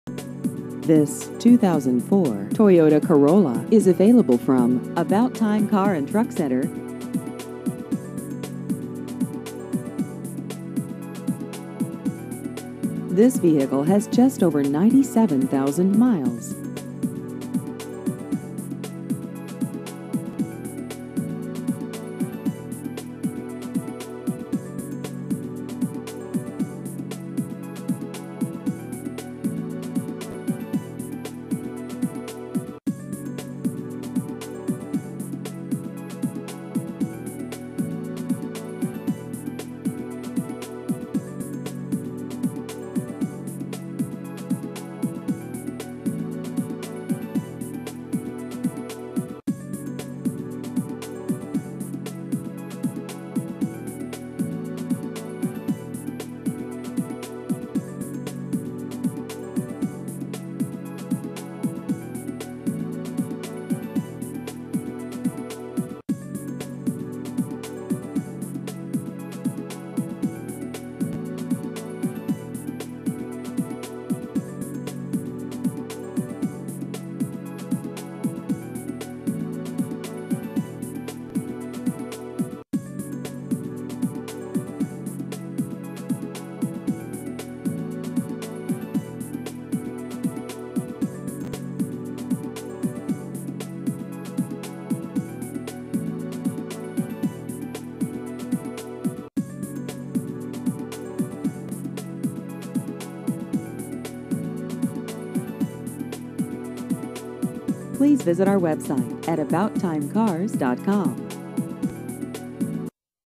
2004 Toyota Corolla Used Cars Englewood CO
This 2004 Toyota Corolla is available from About Time Car and Truck Center.